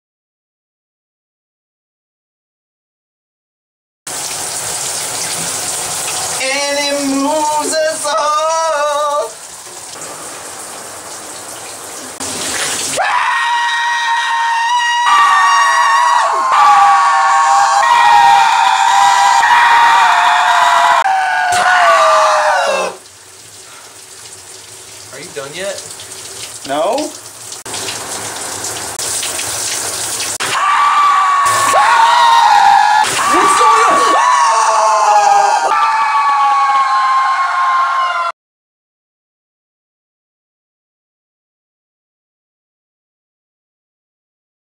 The Shower movie
a shower intrusion goes very bad...